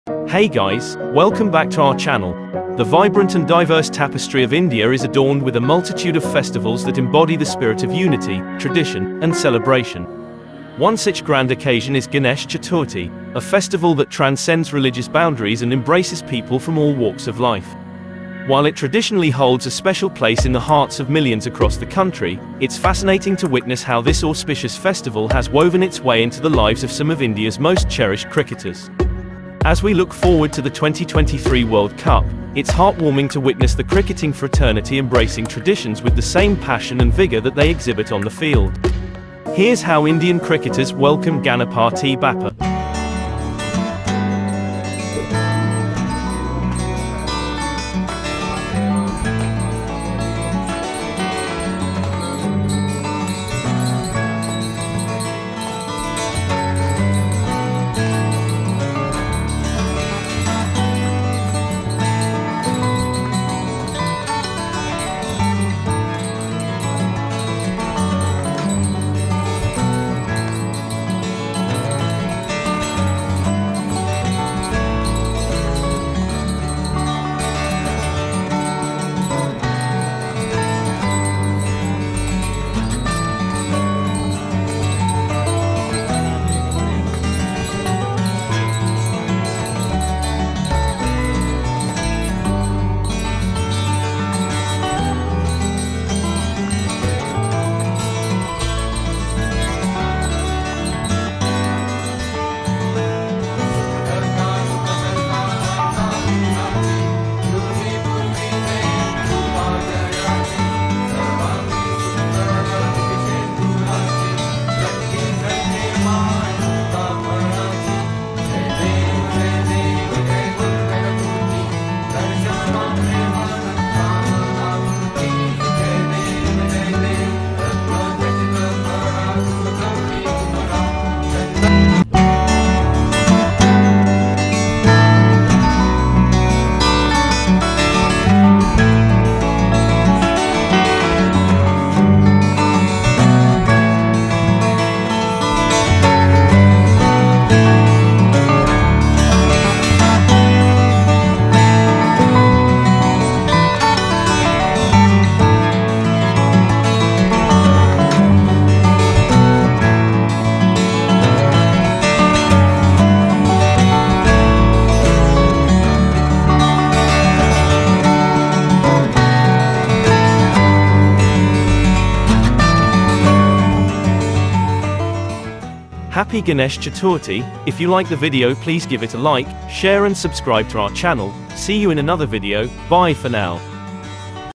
Here’s How Indian Cricketers Welcomed Ganpati Bappa
Get ready to witness the heartwarming and joyous celebration of Ganpati Bappa by our beloved Indian cricketers! 🇮🇳🏏 In this special video, we bring you an exclusive glimpse of how our cricket stars welcomed the elephant-headed God with open arms and immense devotion.

From Virat Kohli's traditional aarti to Rohit Sharma's vibrant Ganpati decoration, this video captures the essence of unity and festivity as our cricket heroes come together to celebrate the arrival of Lord Ganesha. 🎉🕉️

Join us as we take you on a journey filled with music, dance, and the vibrant colors of India's favorite festival. Don't miss out on the heartwarming moments shared by our cricket legends as they seek the blessings of Ganpati Bappa for success and prosperity both on and off the field. 🌟🙌

Hit that "LIKE" button if you're a cricket and Ganpati Bappa enthusiast, and be sure to "SUBSCRIBE" to our channel for more heart-touching moments and exclusive content from the world of sports and culture. 📢🔔